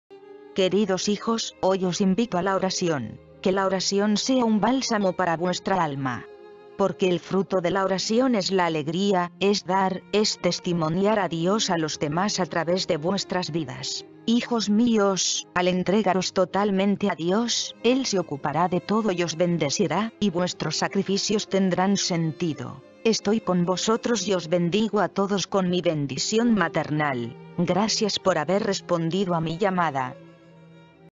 Mensajes de la Virgen Medjugorje, Mensaje del 25 de octubre de 2019
”Queridos hijos, hoy os invito a la oración.
 ¡Que la oración sea un bálsamo para vuestra alma!
 porque el fruto de la oración es la alegría, es dar,
 es testimoniar a Dios a los demás a través de vuestras vidas.
 Hijos míos, al entregaros totalmente a Dios,
Él se ocupará de todo y os bendecirá,
 y vuestros sacrificios tendrán sentido.
 Estoy con vosotros y os bendigo a todos
con mi bendición maternal.
¡Gracias por haber respondido a mi llamada!”

Messages of the Virgin Medjugorje, Message of October 25, 2019

”Dear children, today I invite you to prayer.
  May prayer be a balm for your soul!
  because the fruit of prayer is joy, is to give,
  It is to witness to God to others through your lives.
  My children, by giving yourselves totally to God,
He will take care of everything and bless you,
  and your sacrifices will make sense.
  I am with you and I bless you all
With my motherly blessing.
Thanks for recived my call!"

Messages de la Vierge Medjugorje, Message du 25 octobre 2019

”Chers enfants, aujourd'hui je vous invite à la prière.
  Que la prière soit un baume pour ton âme!
  parce que le fruit de la prière est joie, c'est donner,
  C'est témoigner de Dieu aux autres à travers vos vies.
  Mes enfants, en vous donnant totalement à Dieu,
Il s'occupera de tout et vous bénira,
  et vos sacrifices auront un sens.
  Je suis avec vous et je vous bénis tous
Avec ma bénédiction maternelle.
Merci d'avoir répondu à mon appel! "

Messaggi della Vergine Medjugorje, Messaggio del 25 ottobre 2019

"Cari figli, oggi vi invito alla preghiera.
  Possa la preghiera essere un balsamo per la tua anima!
  perché il frutto della preghiera è la gioia, è dare,
  È testimoniare Dio agli altri attraverso le tue vite.
  Figli miei, dandovi totalmente a Dio,
Si prenderà cura di tutto e ti benedirà,
  e i tuoi sacrifici avranno senso.
  Sono con te e ti benedico tutti
Con la mia benedizione materna.
Grazie per aver risposto alla mia chiamata! ”